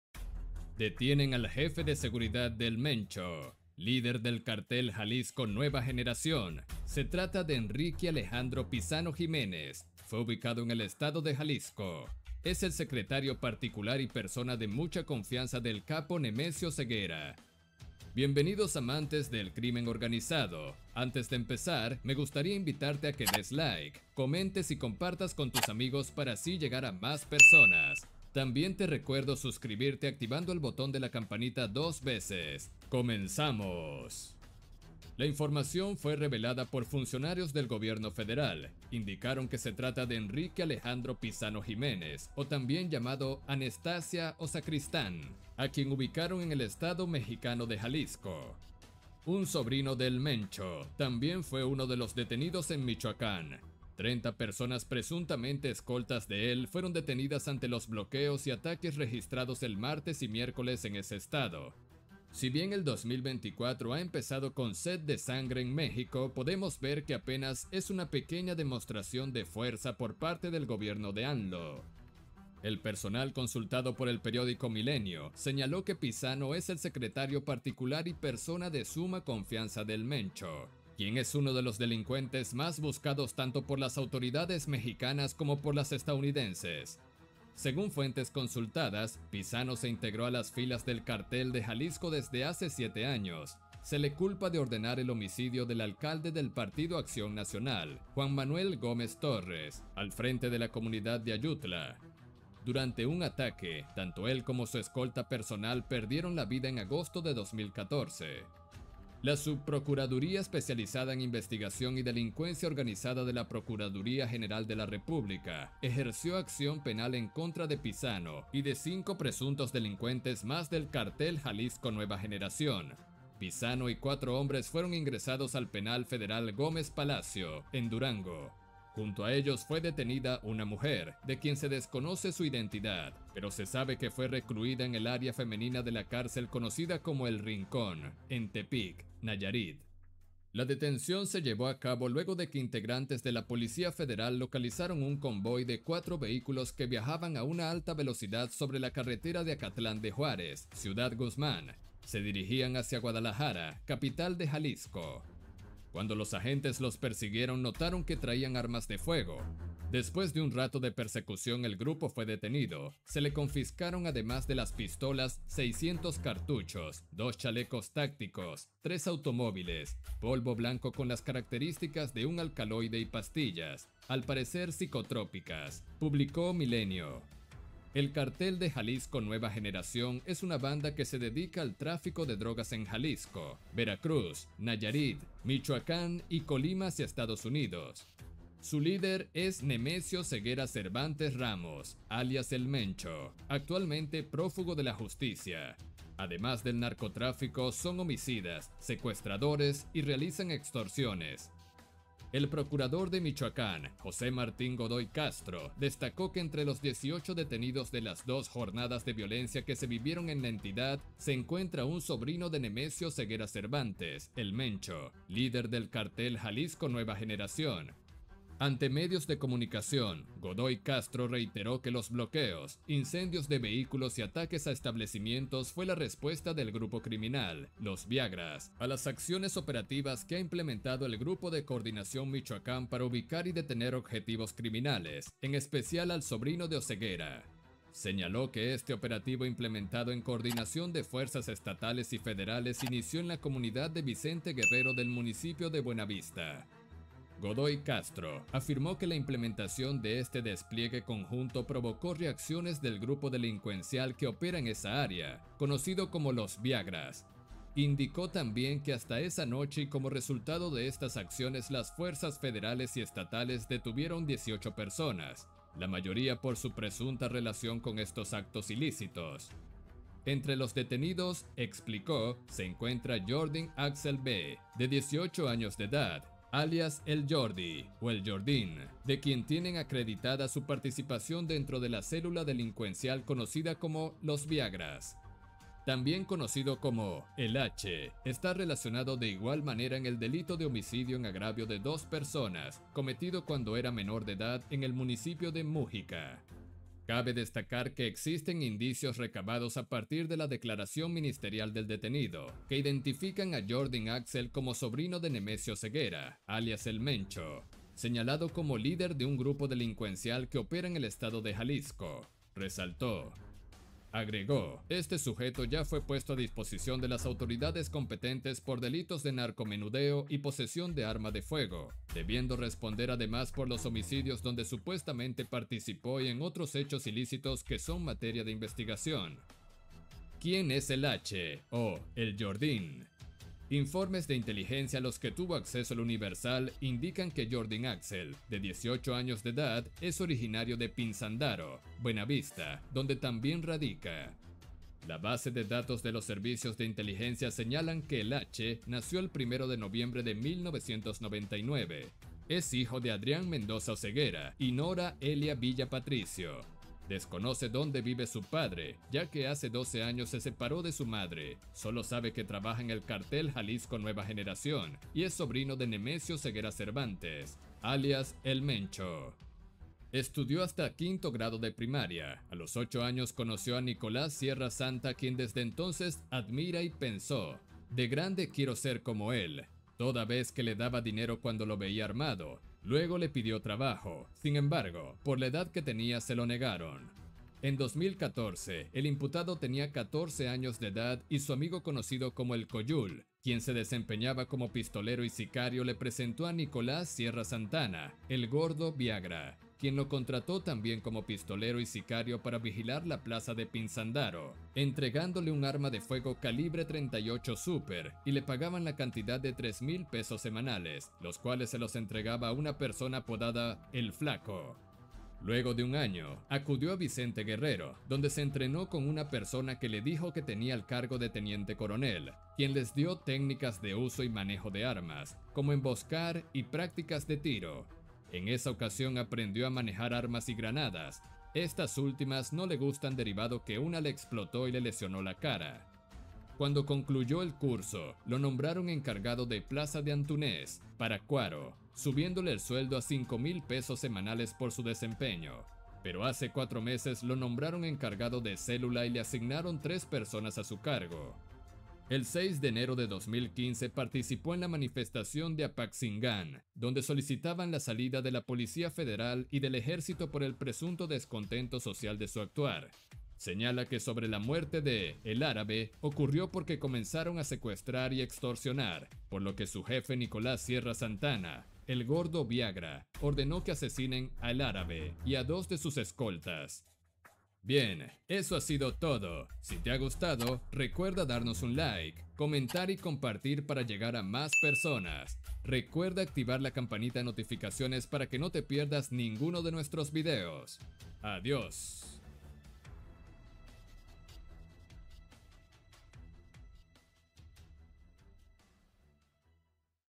🚨¡ÚLTIMA HORA! Detienen A 36 Escoltas Del MENCHO En Michoacán En Un Topon
APOYANOS DEJANDO UN COMENTARIO Y UN BUEN LIKE.

Un sobrino de “El Mencho”, también fue uno de los detenidos en Michoacán 30 personas presuntamente escoltas de él fueron detenidas ante los bloqueos y ataques registrados el martes y miércoles en ese estado, si bien el 2024 ha empezado con sed de sangre en México podemos ver que apenas es una pequeña demostración de fuerza por parte del gobierno de AMLO